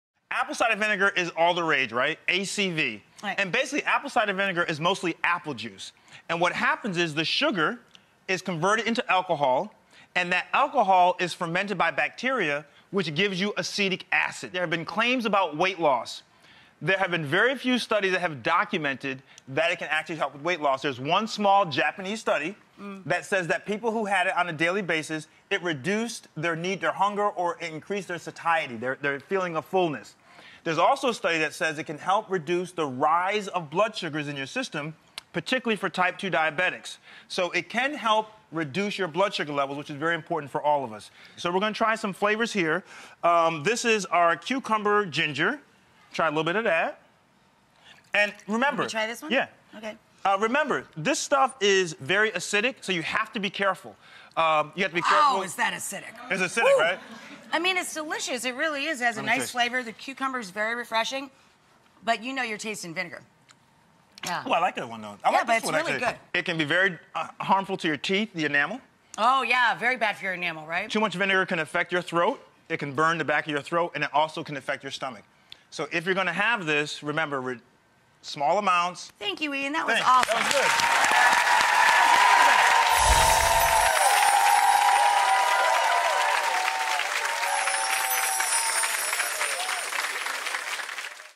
Can Apple Cider Vinegar Actually Help You Lose Weight?
Can this tart liquid actually help you drop pounds? "Blast the Sugar Out" author Dr. Ian spills the tea.